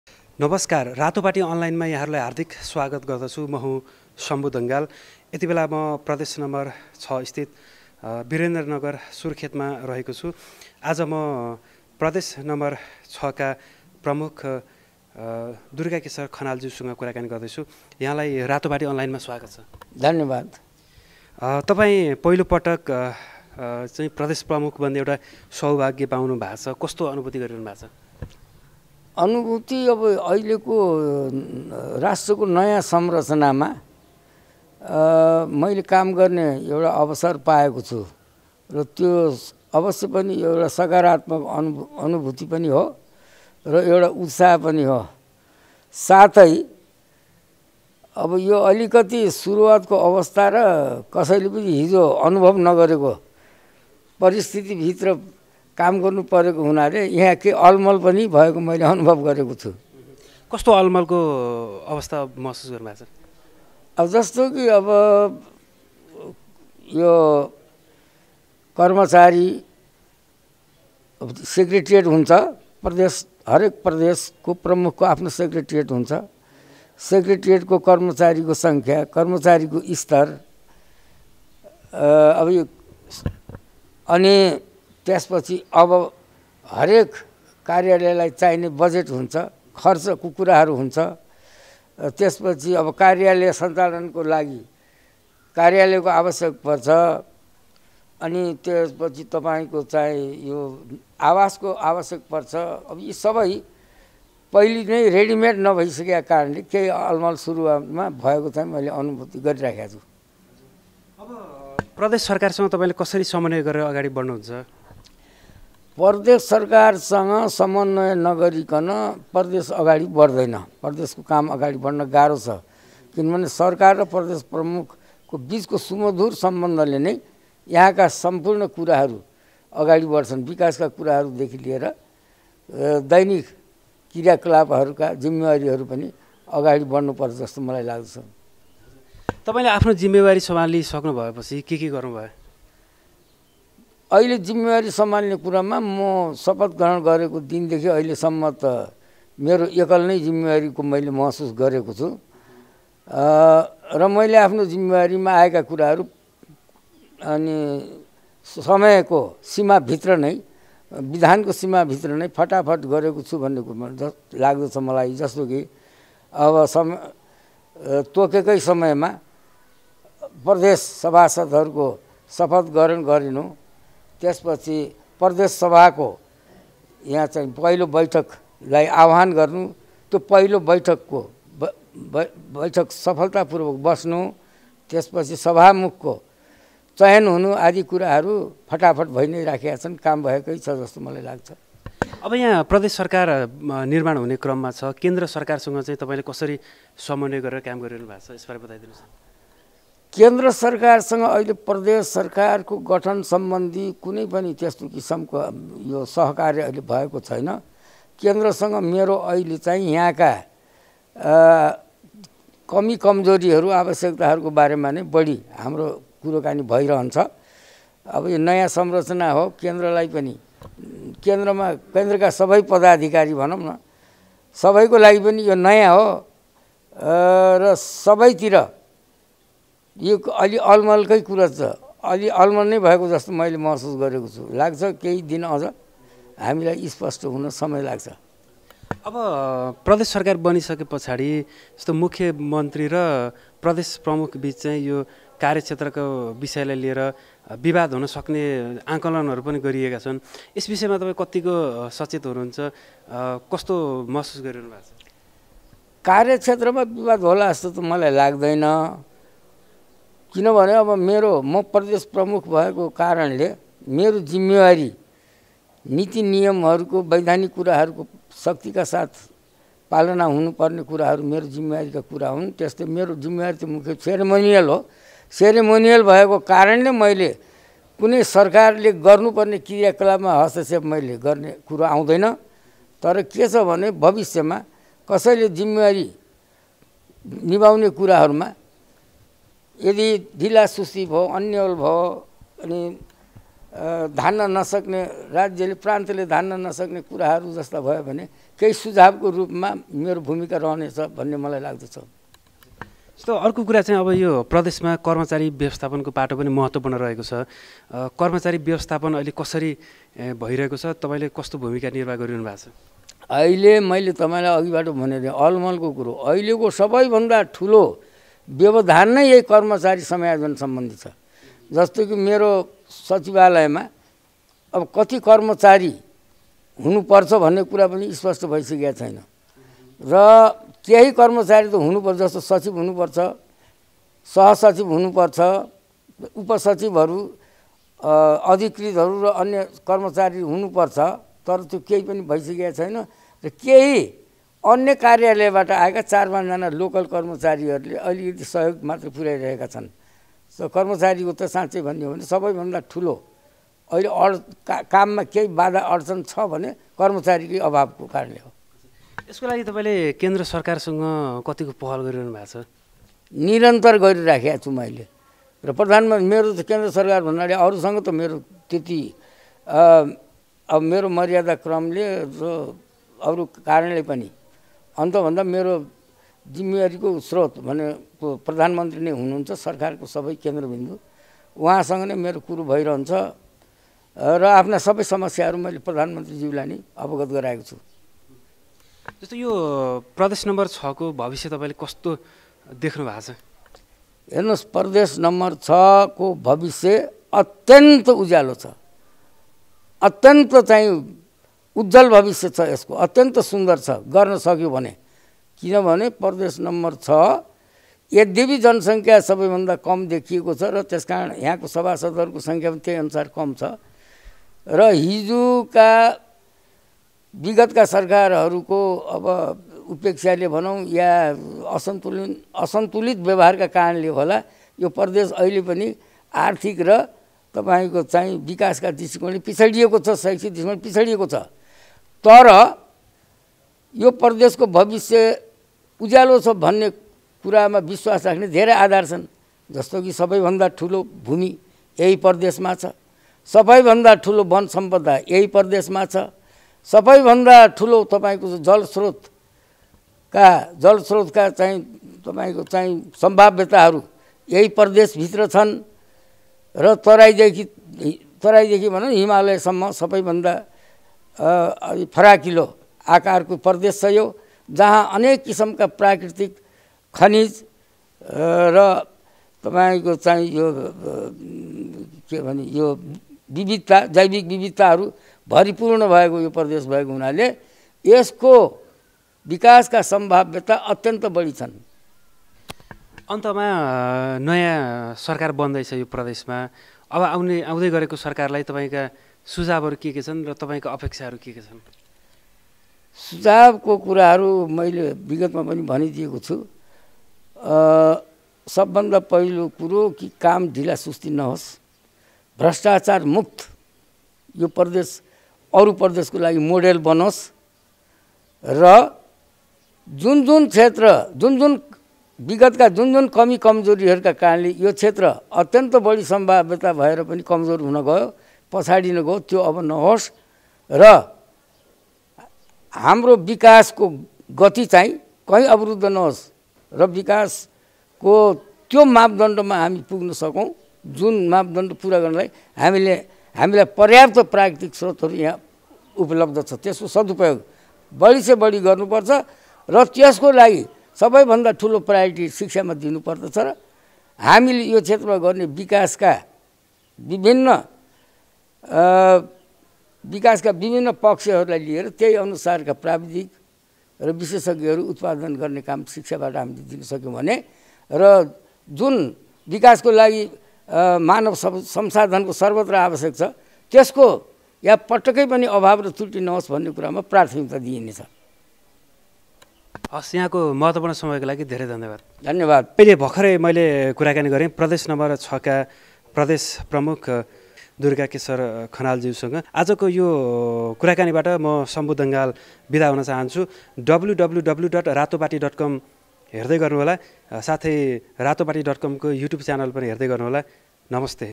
प्रदेश नं. ६ का प्रमुख दुर्गाकेशर खनाल– ‘नयाँ सिस्टममा सबै कुरा अलमल भयो’
Interview with Durga keshar khanal